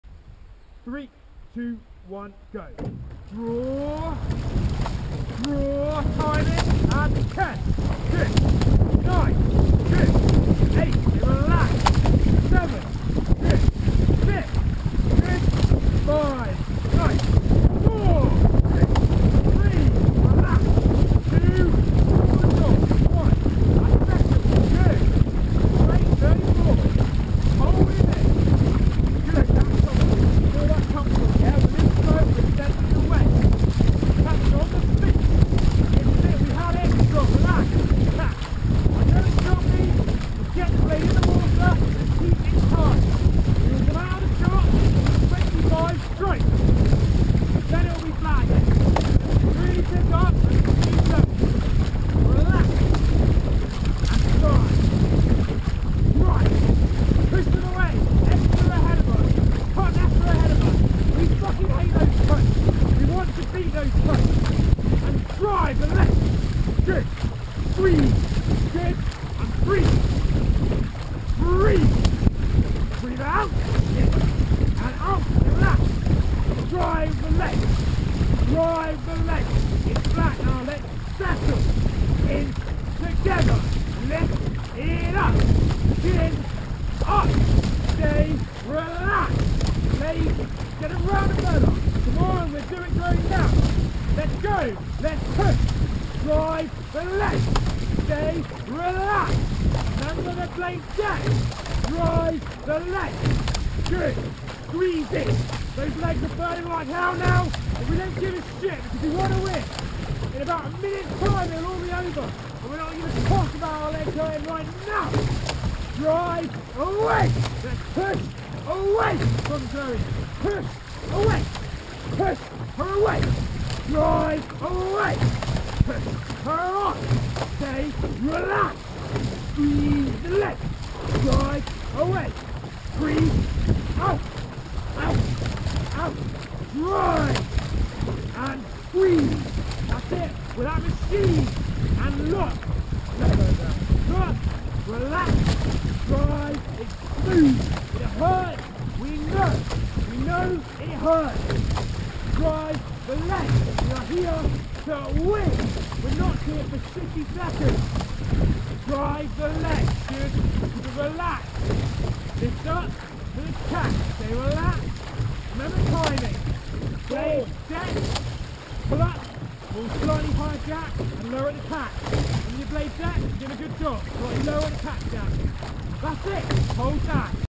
How to be a shouty cox.
Mens 4+ race rehearse a 1km.